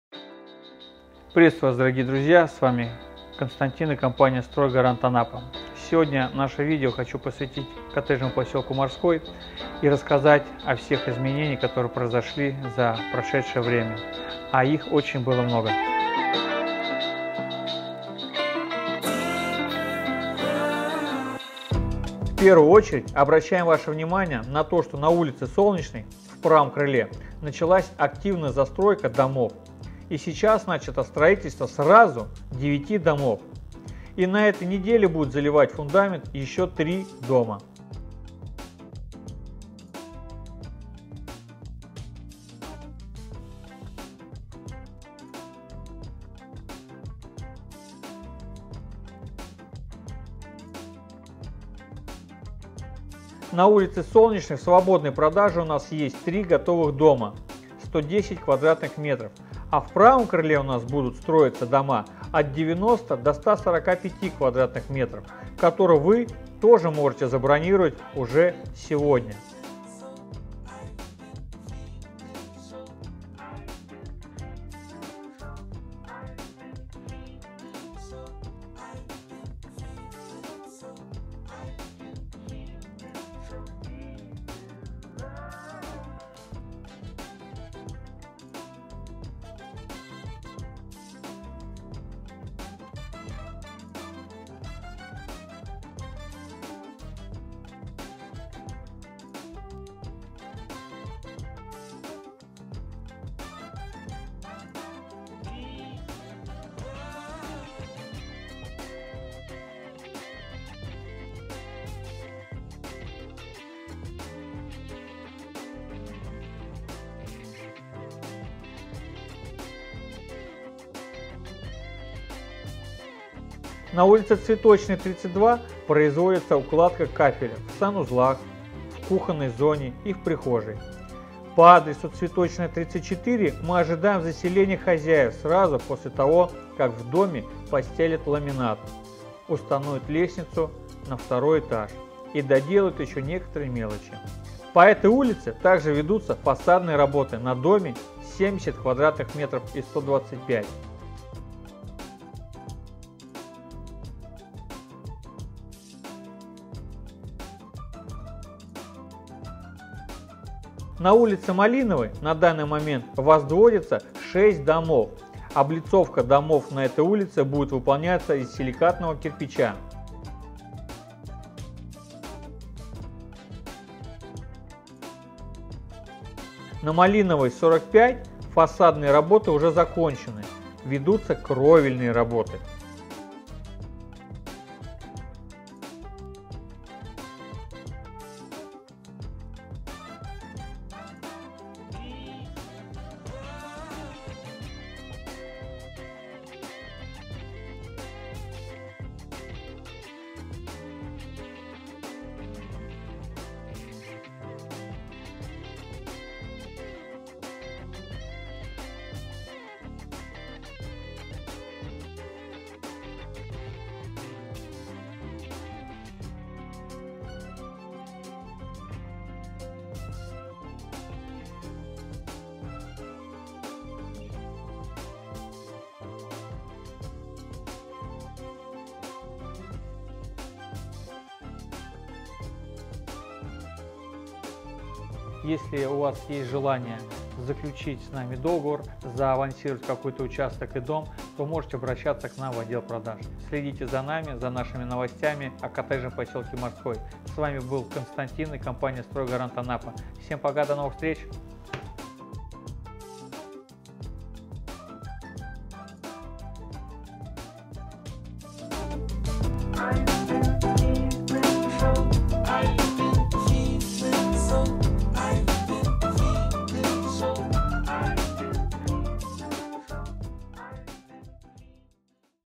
Новые дома под ключ на побережье. Дом в Анапе. Купи дом своей мечты уже сегодня!
Рассказываем о новостях строительства в коттеджном поселке Морской в 4х км от моря. Уже сегодня вы можете приобрести дом своей мечты на побережье Черного моря вблизи города-курорта Анапы.